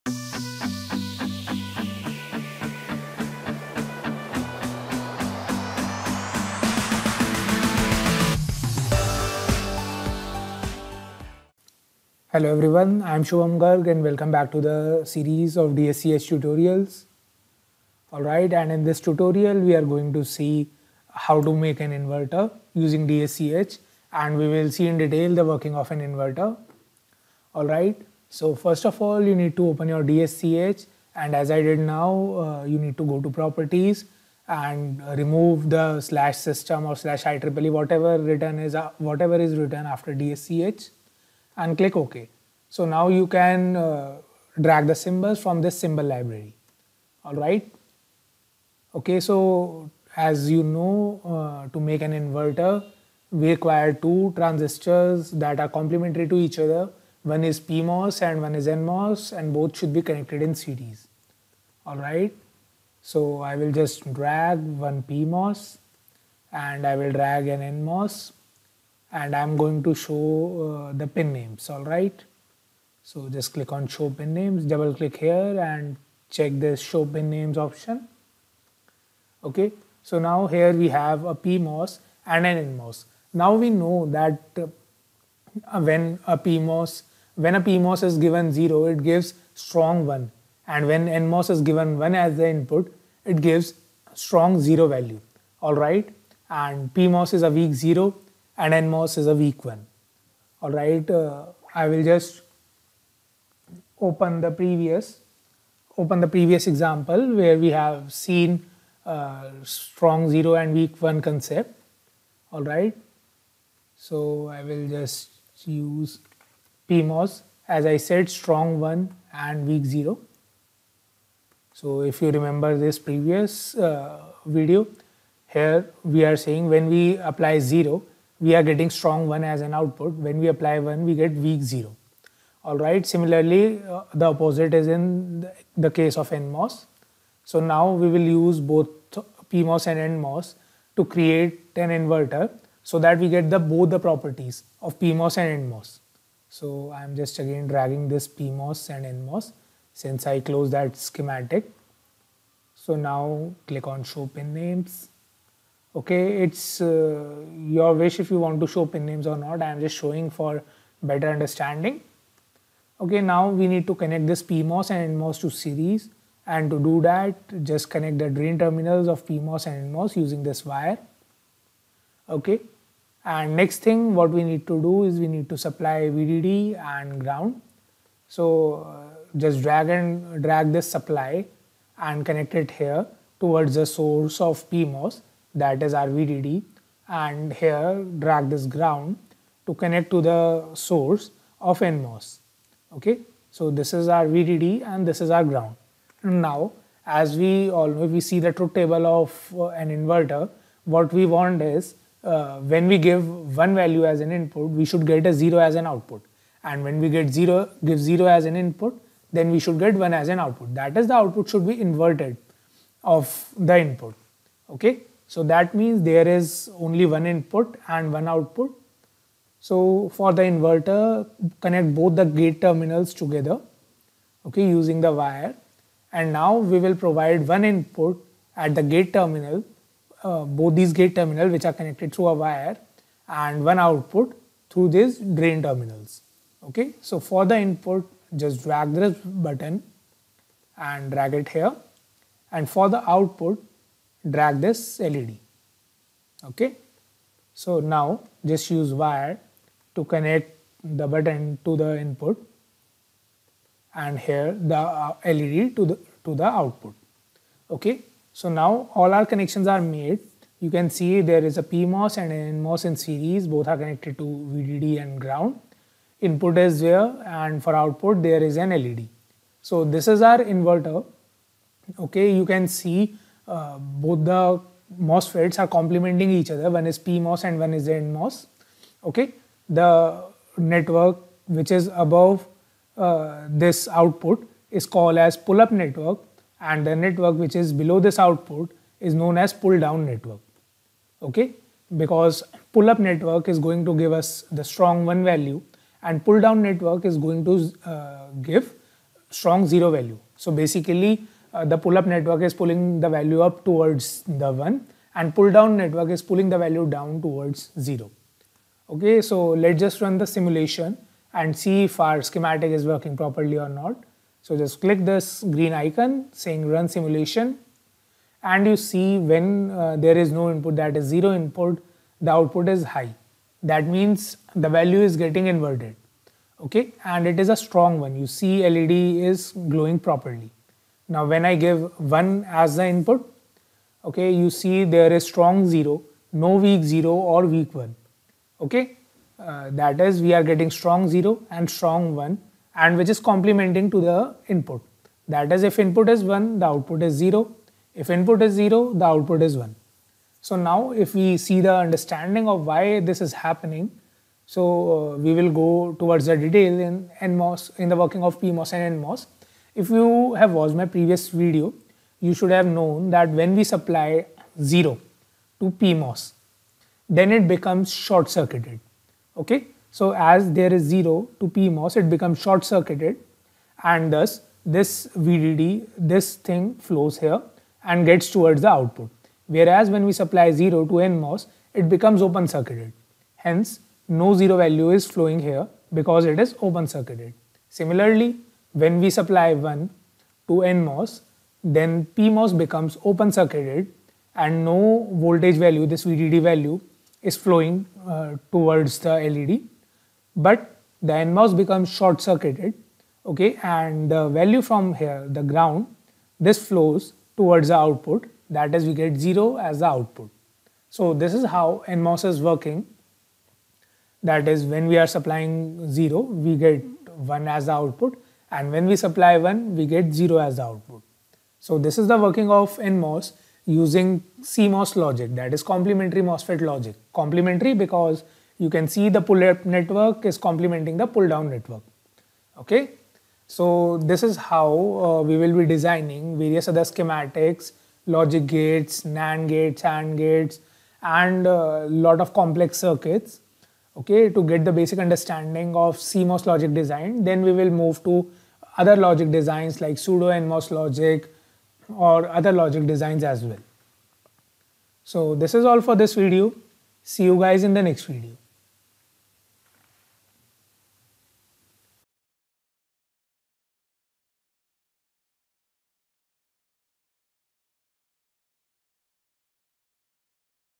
Inverter Schematic and Simulation using DSCH
In this Video we will see how to design and simulate the schematic of an Inverter using DSCH Software.